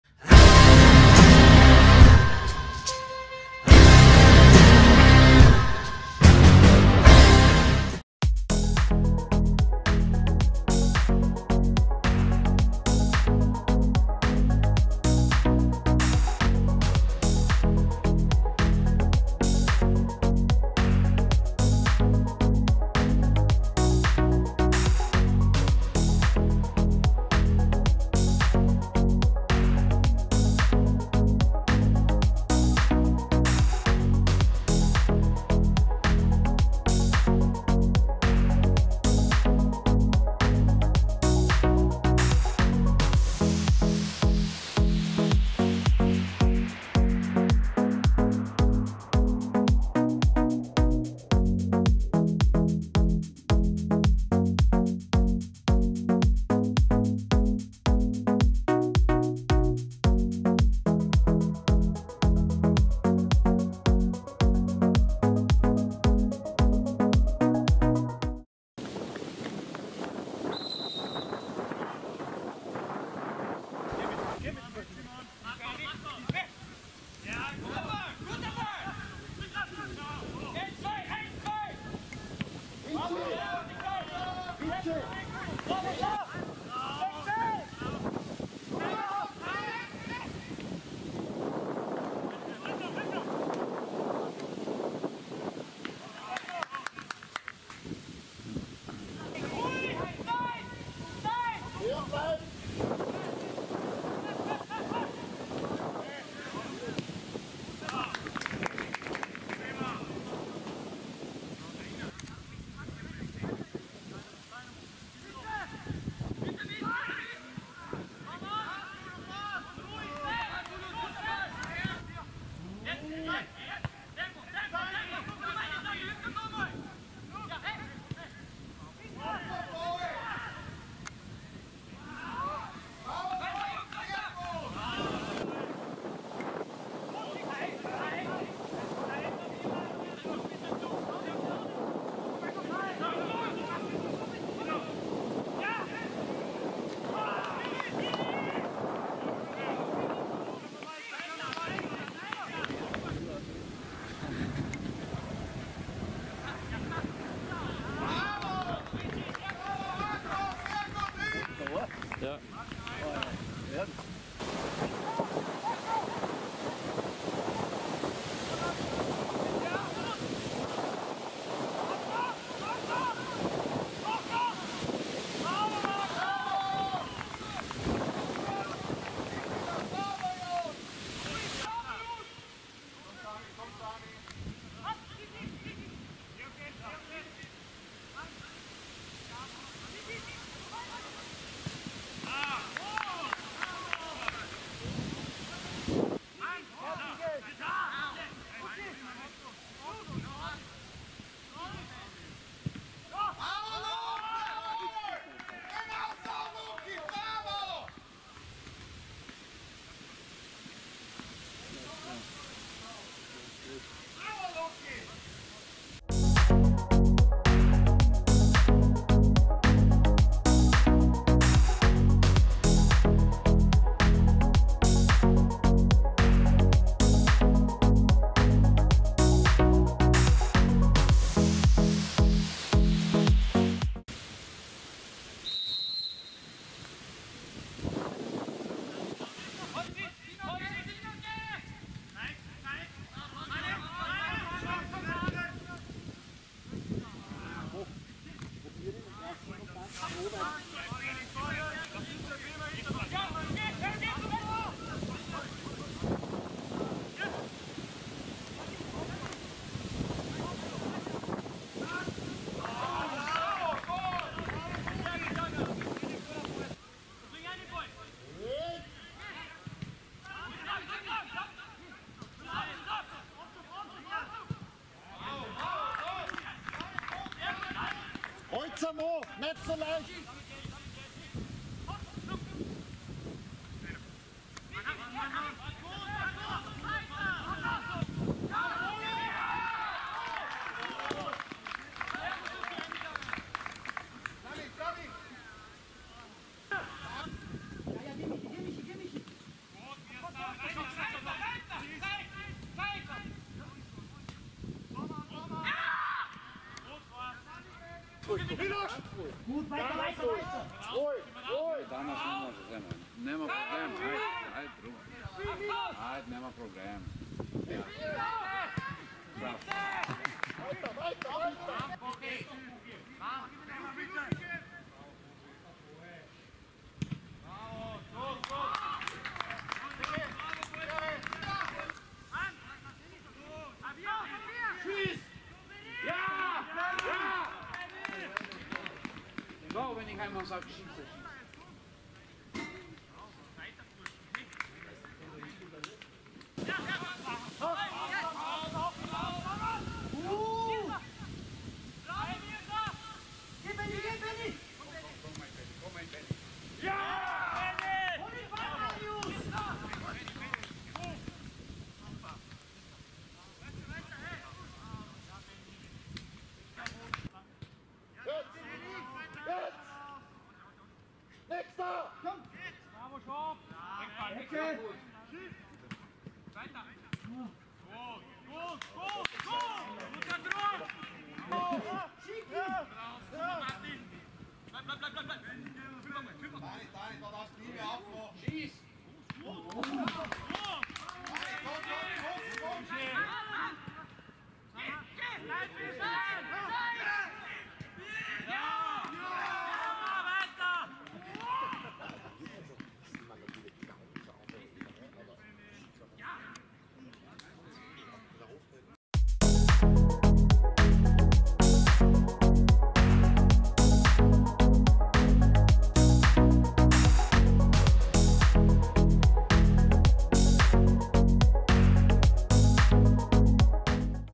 ⚫️ HIGHLIGHTS KM "Testspiel" ⚽️ ASKÖ-Murfeld 🆚 ESK Graz ⚽️ vom 26.07.2021
▶️ HIGHLIGHTS Freundschaftsspiel zur Vorbereitung ◀️
▶️ Sportzentrum ASKÖ-Murfeld ◀️
▶️ Endstand 4:3 (3:0) ◀️
Tor ESK Graz:
2 x

Tore ASKÖ-Murfeld:
Richard Macher, Marko Radosevic, Lukas Macher, Muhammet Bulut